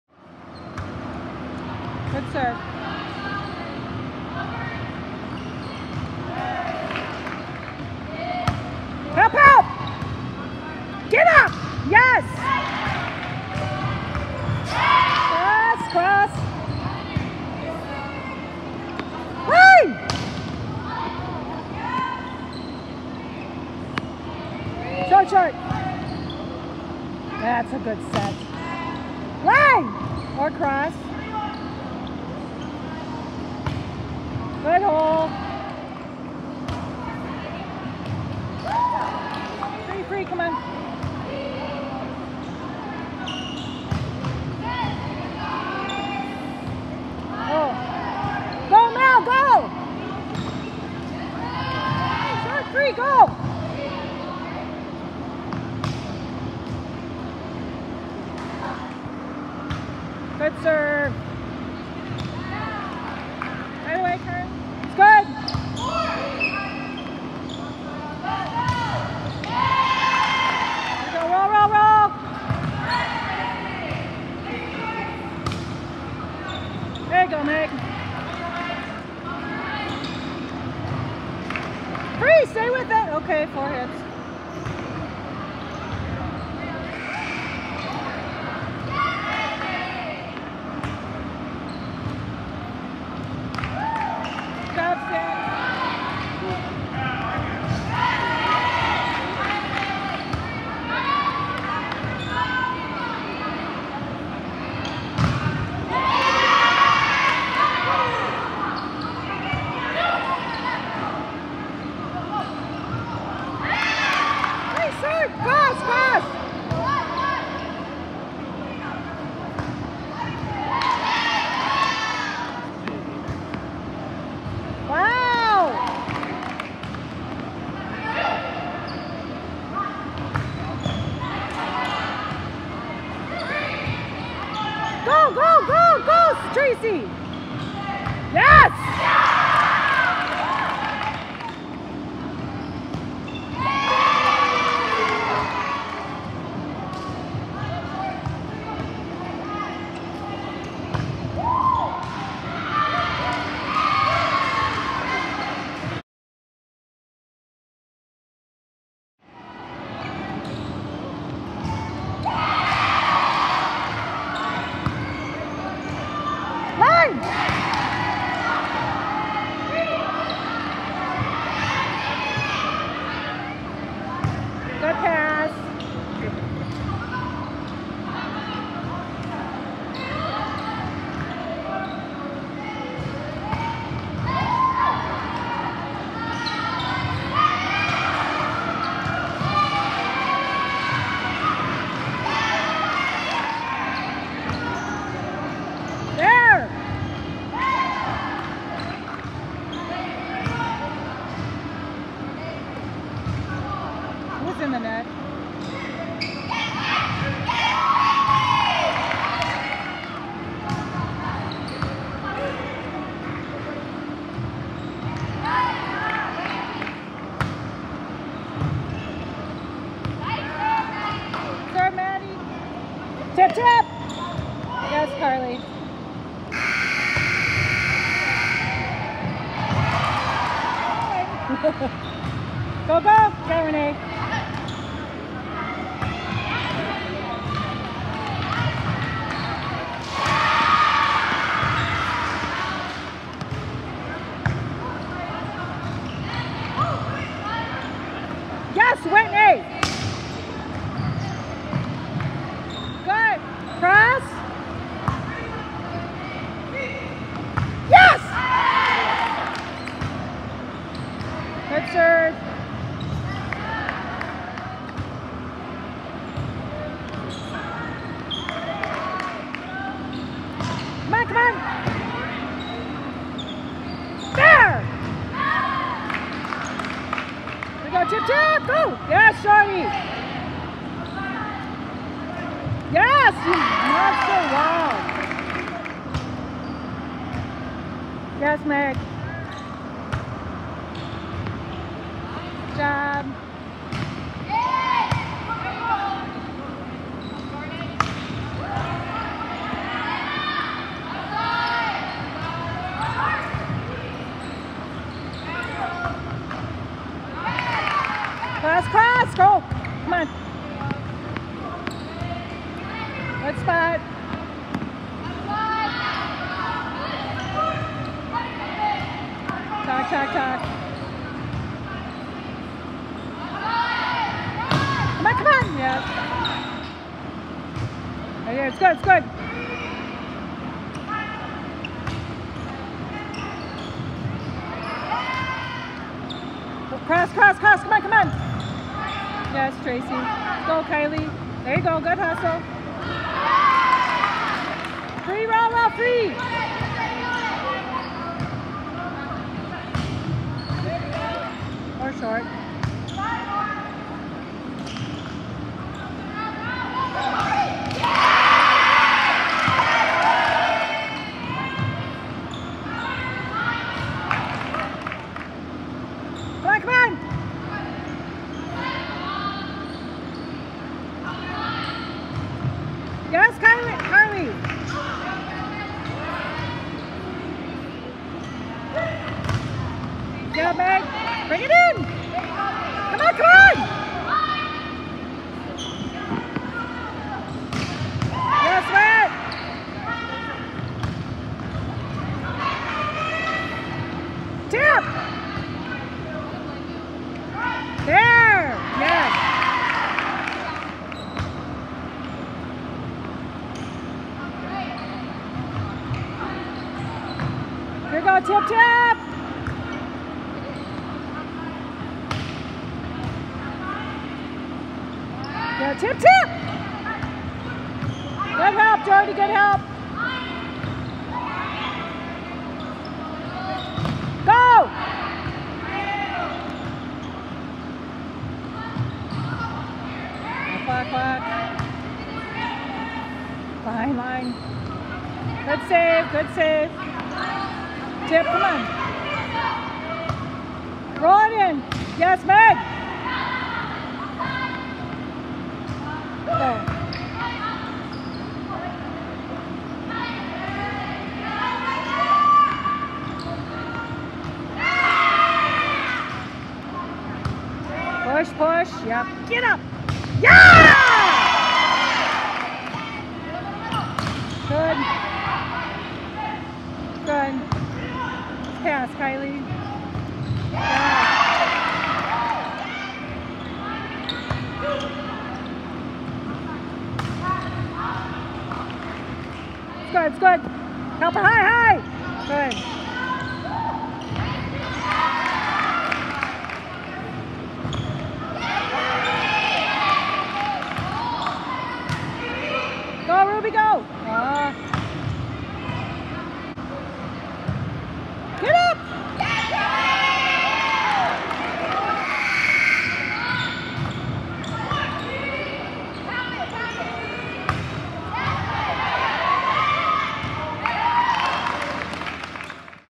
2022 Grantsburg VB vs. Shell Lake - 8.20 scrimmage highlights
No officials, no scores, just pre-season VB. Mute the audio to avoid random outbursts from the coach.